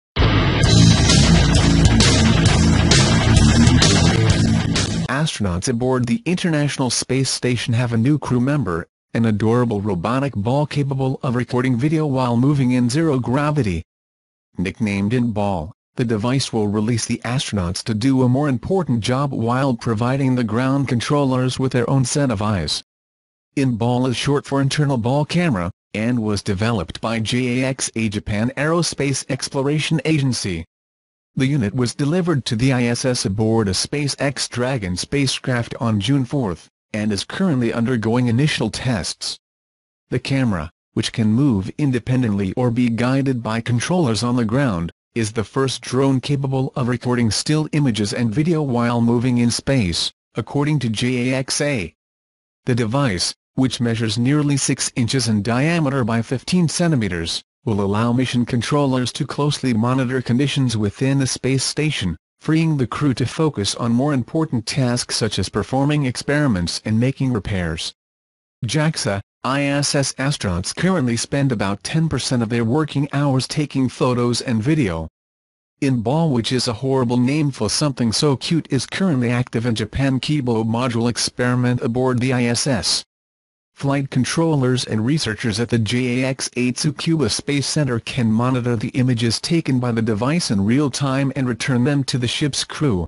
This floating robotic camera is the most beautiful thing ever sent to space.
Astronauts aboard the International Space Station have a new crew member: an adorable robotic ball capable of recording video while moving in zero gravity. Nicknamed Int Ball, the device will release the astronauts to do a more important job while providing the ground controllers with their own set of eyes. Int Ball is short for Internal Ball Camera, and was developed by JAXA Japan Aerospace Exploration Agency. The unit was delivered to the ISS aboard a SpaceX Dragon spacecraft on June 4, and is currently undergoing initial tests. The camera, which can move independently or be guided by controllers on the ground, is the first drone capable of recording still images and video while moving in space, according to JAXA.

The device, which measures nearly six inches in diameter by 15 cm, will allow mission controllers to closely monitor conditions within the space station, freeing the crew to focus on more important tasks such as performing experiments and making repairs. JAXA, ISS astronauts currently spend about 10 percent of their working hours taking photos and video. Int-Ball which is a horrible name for something so cute is currently active in Japan Kibo "module experiment aboard the ISS. Flight controllers and researchers at the JAXA Tsukuba Space Center can monitor the images taken by the device in real time And return them to the ship's crew.